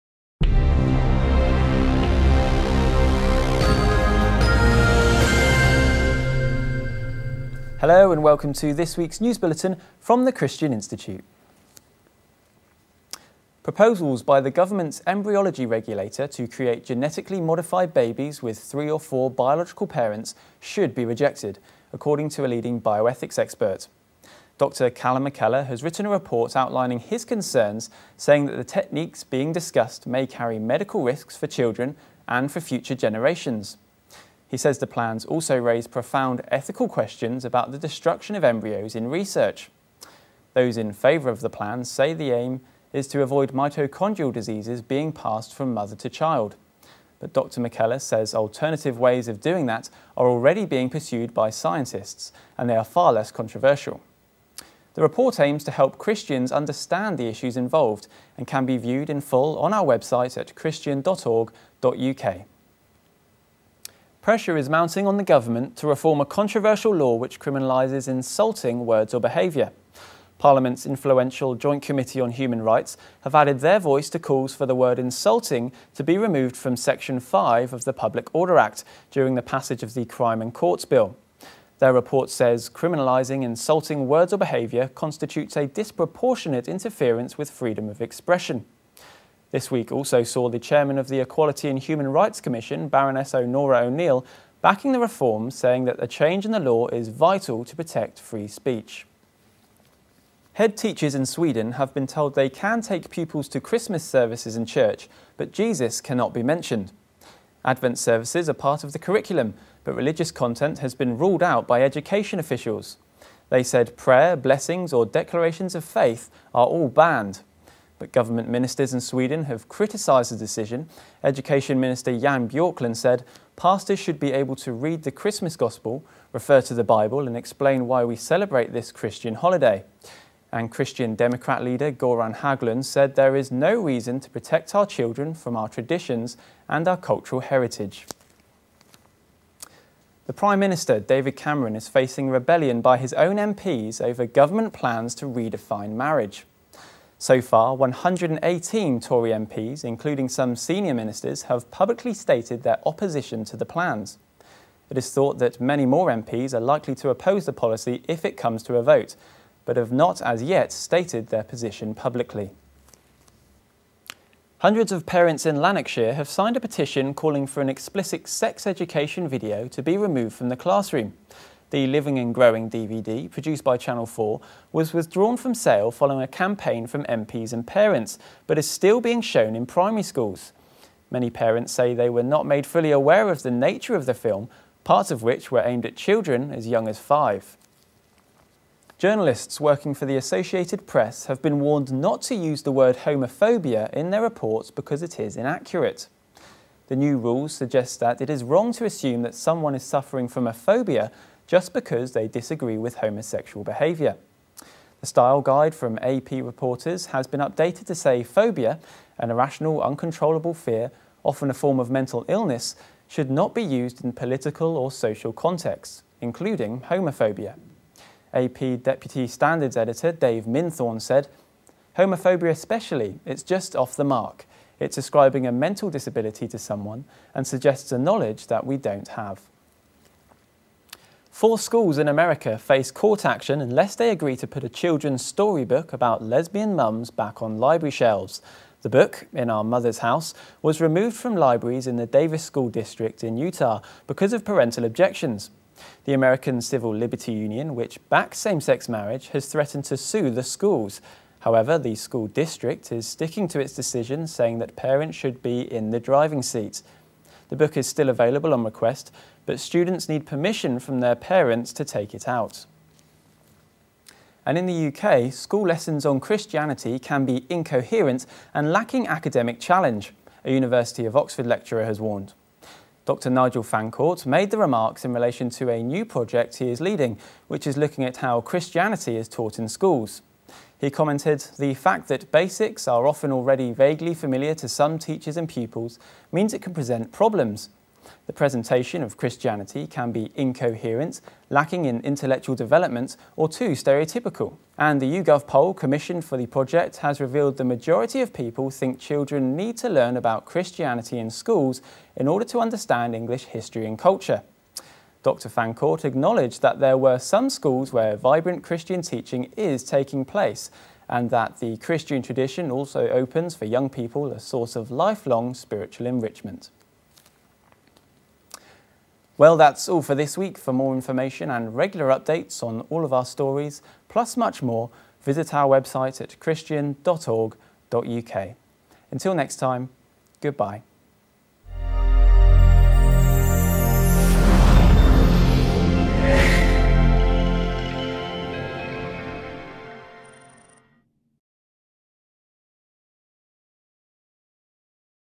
News Bulletin 30 November 2012 -- The Christian Institute
Proposals by the Government's embryology regulator to create genetically modified babies with three or four biological parents, should be rejected, according to Dr Calum MacKellar, a leading bioethics expert -- Pressure is mounting on the Government to reform a controversial law which criminalises "insulting" words or behaviour -- Head teachers in Sweden have been told they 'can' take pupils to Christmas services in church - but Jesus 'cannot' be mentioned -- The Prime Minister David Cameron is facing a rebellion by 118 of his own MPs over plans to redefine marriage -- Hundreds of parents in Lanarkshire have signed a petition calling for an explicit sex education DVD to be removed from the classroom -- Journalists working for the Associated Press have been warned not to use the word 'homophobia' in their reports because it is inaccurate -- Four schools in America face court action unless they agree to put a children's storybook about lesbian mums back on library shelves -- And in the UK, School lessons on Christianity can be incoherent, and lacking academic challenge a University of Oxford lecturer has warned.